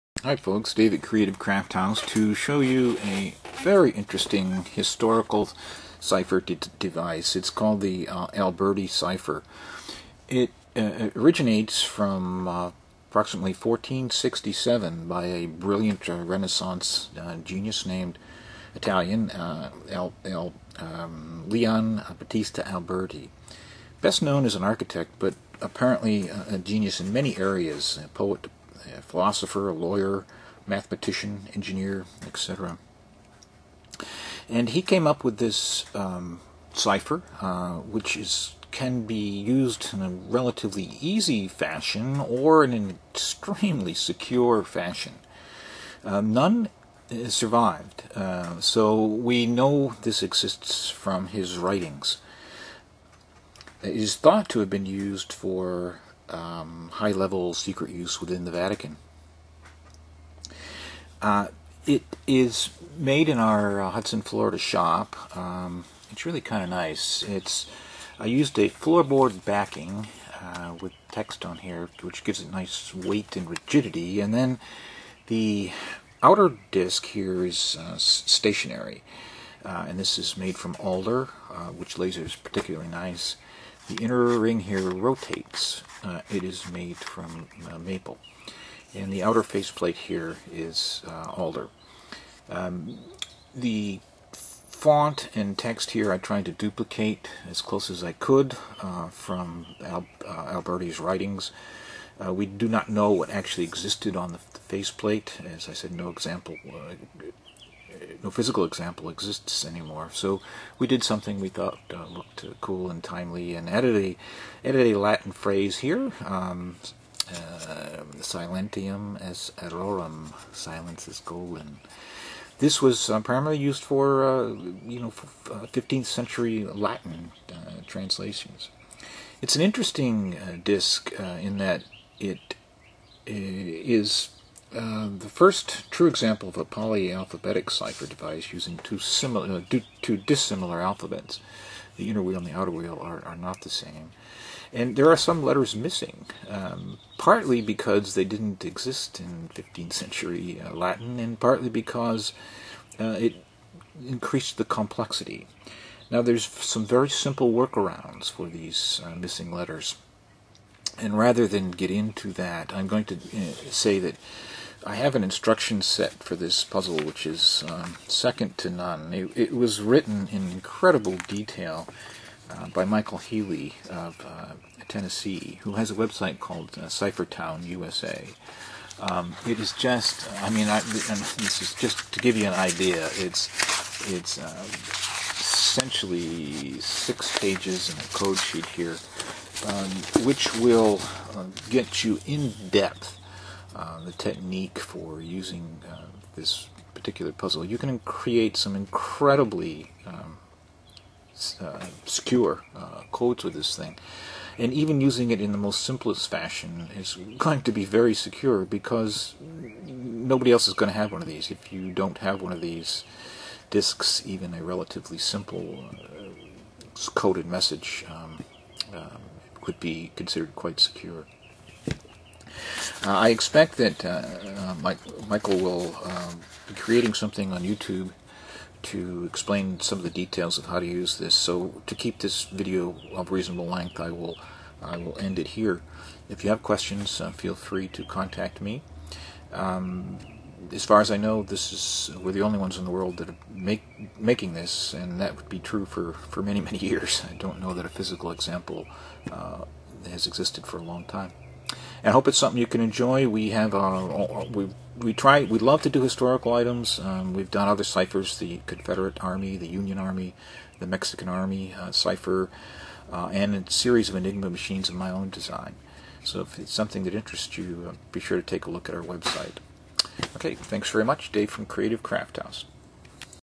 Alberti Cipher
The Alberti Cipher Disk is the first example of a true “polyalphabetic” cipher device using two dissimilar alphabets.  It can be postulated from Alberti's writings that this cipher device may have been created for high level secret use within the Vatican.

The Alberti Cipher Disk was invented by the famous Italian scholar Leon Battista Alberti in 1467.  Alberti was an amazingly brilliant Renaissance genius whose talents covered a wide range of fields. He was a renowned architect, a dedicated linguist, a poet, a priest, a philosopher, A lawyer, an astounding mathematician, an engineer, author, astronomer, a master horseman, and a cryptographer!

Device Design:
The Alberti Cipher Disk consists of two disks, the stationary outer, originally called by Alberti - the “Stabilis“, and the rotating inner - which he named the “Mobilis.  This Creative Crafthouse device is a faithful rendition of the original Alberti Cipher Disk design, where the Inner disk turns relative to the Outer.  The Cipher is about 4 ½” inches in Diameter.  All characters are deeply laser engraved into the woods for both beauty and durability.

The disk comes with an incredibly complete set of instructions on various techniques  to use the cipher.   These instructions were written by Michael Healey of TN  (his website is CIPHERTOWN.COM).    There are ways to use the disk easily with modest levels of security and more complex ways to use the disk such that breaking the code would be a challenge for the best of modern day cryptographers using computers.  All are explained in the instruction set which should delight both novice and serious cryptographers.
Made in our Hudson, Florida shop